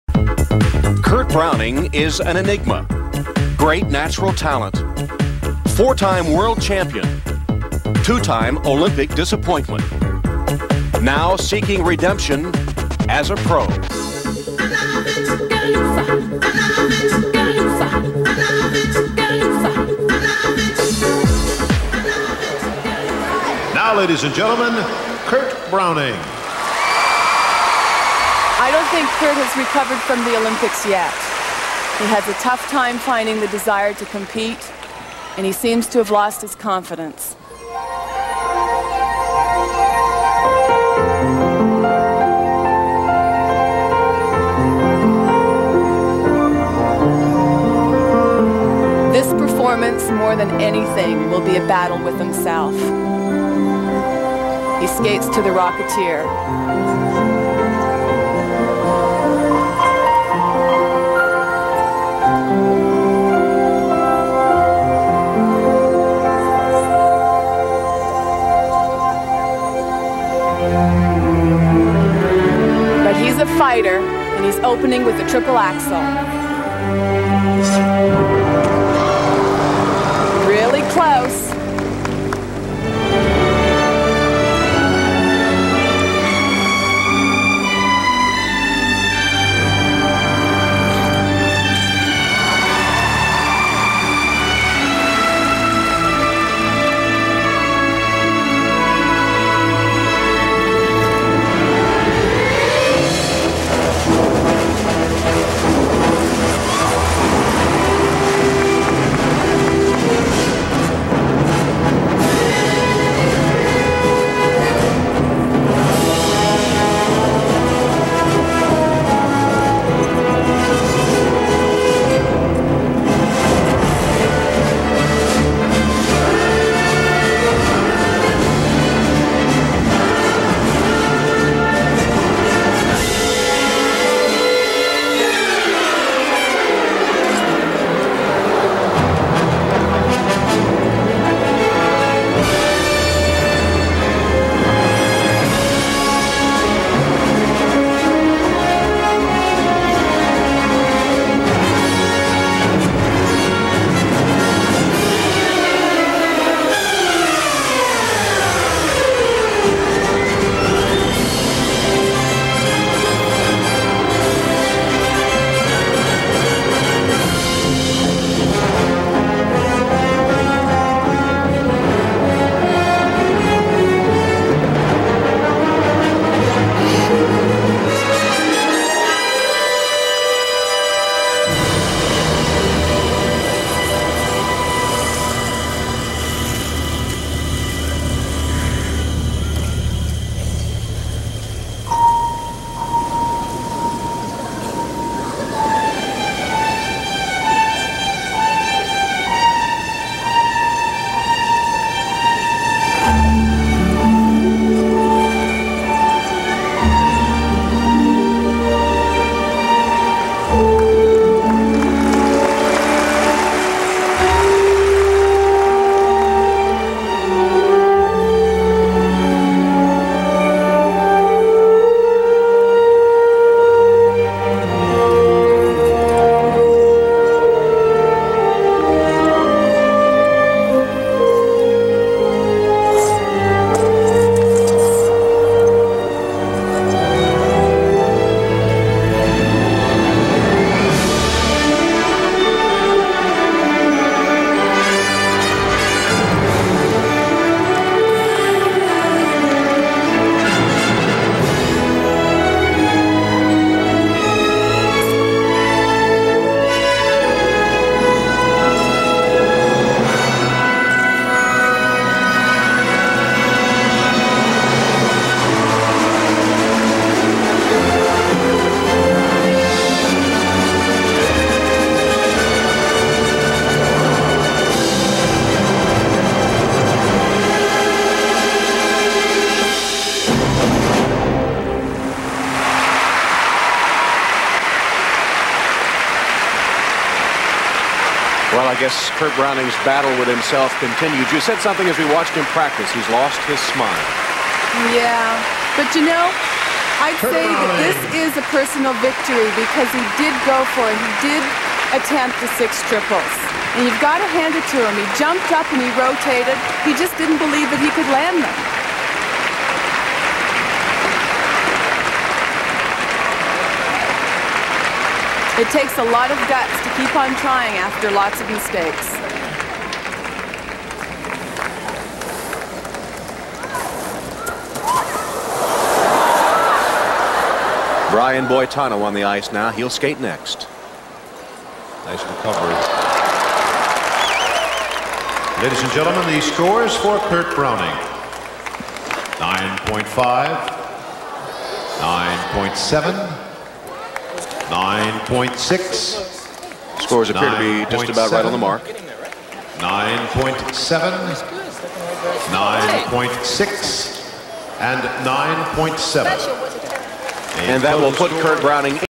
Kurt Browning (CAN) - 1994 World Professionals, Men's' Technical Program
Landover, Maryland, USA - 1994 World Professional Figure Skating Championships, Men's' Technical Program - Kurt Browning of Canada